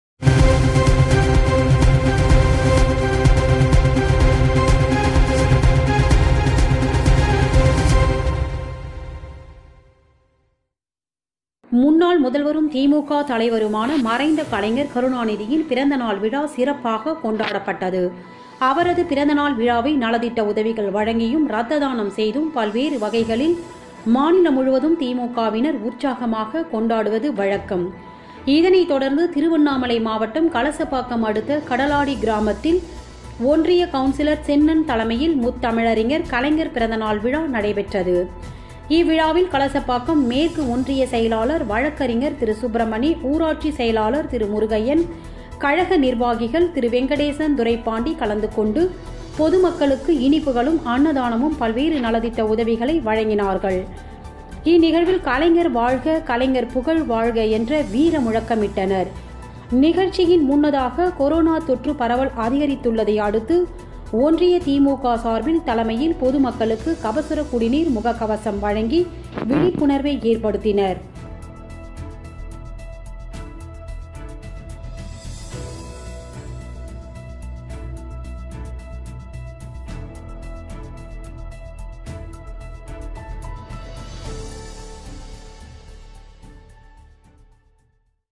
கடலாடி கிராமத்தில் கலைஞர் பிறந்தநாள் விழா எலிமையாக கொண்டாடப்பட்டது.
கடலாடி கிராமத்தில் கலைஞர் பிறந்தநாள் விழா;

 எளிமையாக கொண்டாடப்பட்டது

முன்னாள் முதல்வரும், திமுக தலைவருமான மறைந்த கலைஞர் கருணாநிதியின் பிறந்தநாள் விழா  சிறப்பாக கொண்டாடப்பட்டது அவரது பிறாந்தநாள் விழாவை நலத்திட்ட உதவிகள் வழங்கியும், ரத்த தானம் செய்தும் பல்வேறு வகைகளில் மாநிலம் முழுவதும் திமுகவினர் உற்சாகமாக கொண்டாடுவது வழக்கம். இதனை தொடர்ந்து
திருவண்ணாமலை மாவட்டம் கலசப்பாக்கம் அடுத்த கடலாடி கிராமத்தில் ஒன்றிய கவுன்சிலர் சென்னன் தலைமையில் முத்தமிழறிஞர் கலைஞர் பிறந்தநாள் விழா நடைபெற்றது இவ்விழாவில் கலசப்பாக்கம் மேற்கு ஒன்றிய செயலாளர் வழக்கறிஞர் சுப்பிரமணி ஊராட்சி செயலாளர் முருகையன் கழக நிர்வாகிகள் வெங்கடேசன் துரைப்பாண்டி கலந்து கொண்டு பொதுமக்களுக்கு இனிப்புகளும் அன்னதானமும் பல்வேறு நலத்திட்ட உதவிகளை வழங்கினார்கள் இந்நிகழ்வில் கலைஞர் வாழ்க, கலைஞர் புகழ் வாழ்க, என்ற வீர முழக்கம் இட்டனர். நிகழ்ச்சியின் முன்னதாக கொரோனா தொற்று பரவல் அதிகரித்துள்ளதை அடுத்து  ஒன்றிய திமுக சார்பில் தலைமையில் பொதுமக்களுக்கு கபசுரக் குடிநீர், முக கவசம் வழங்கி விழிப்புணர்வு ஏற்படுத்தினர்